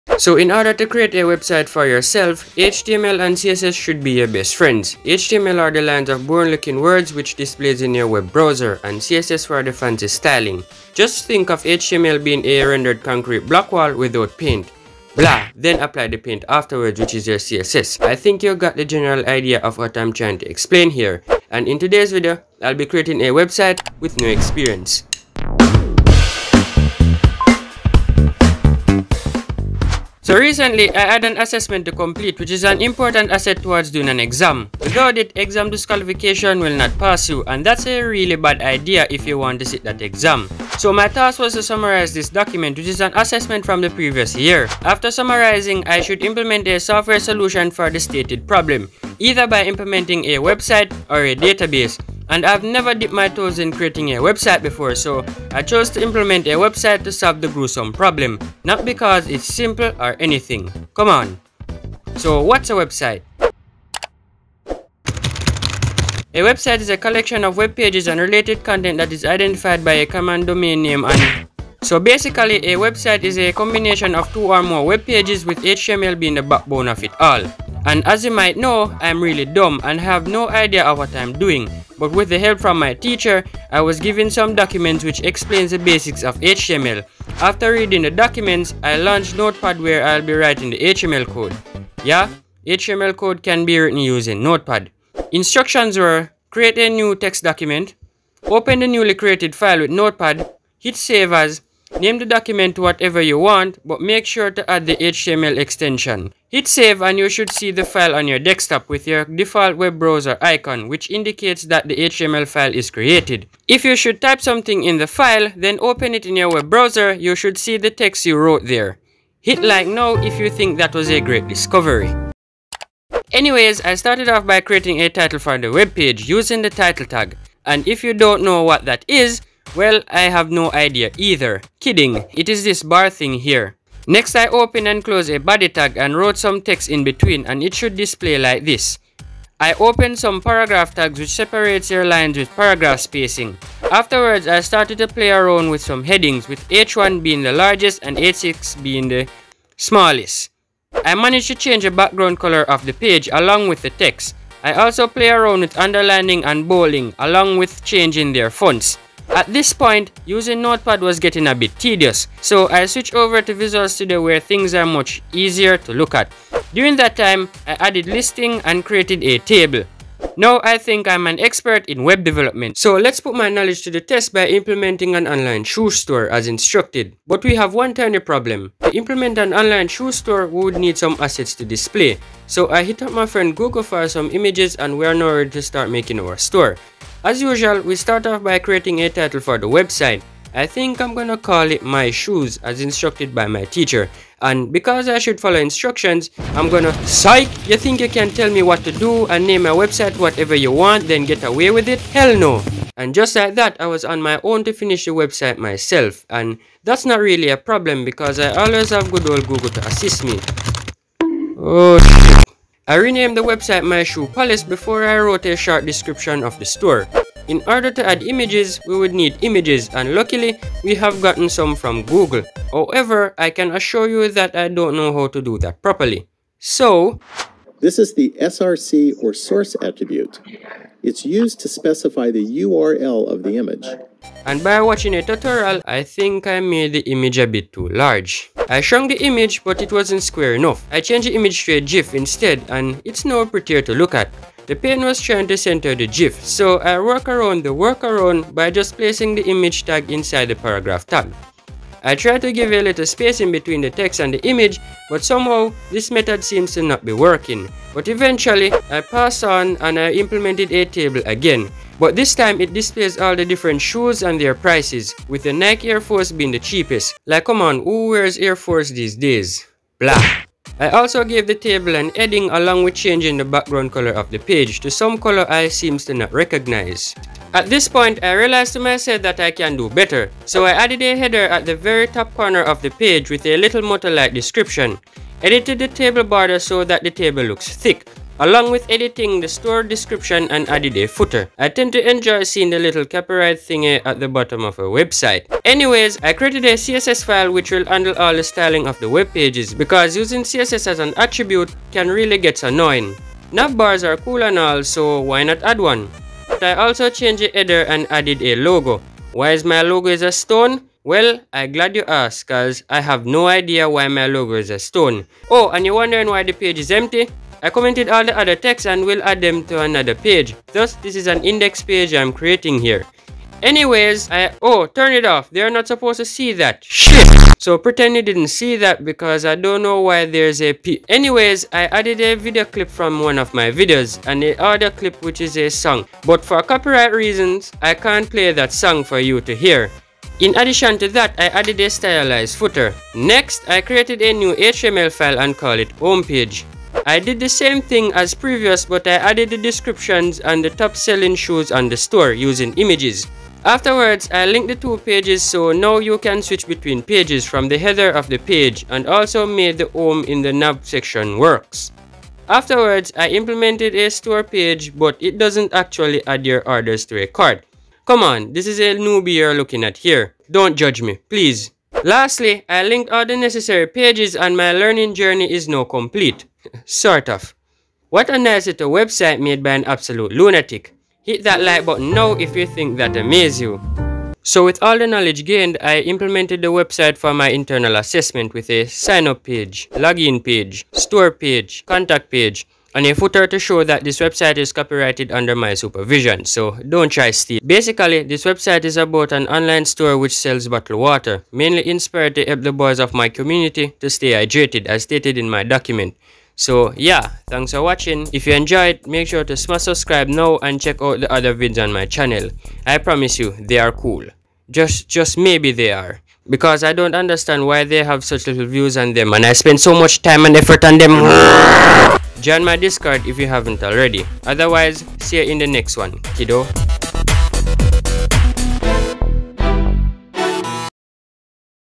I Made a Simple Website with ZERO EXPERIENCE
Well, it has been a long long long while since the last, but I have made a website with no experience with HTML and CSS *years ago* lol

Enjoy! :D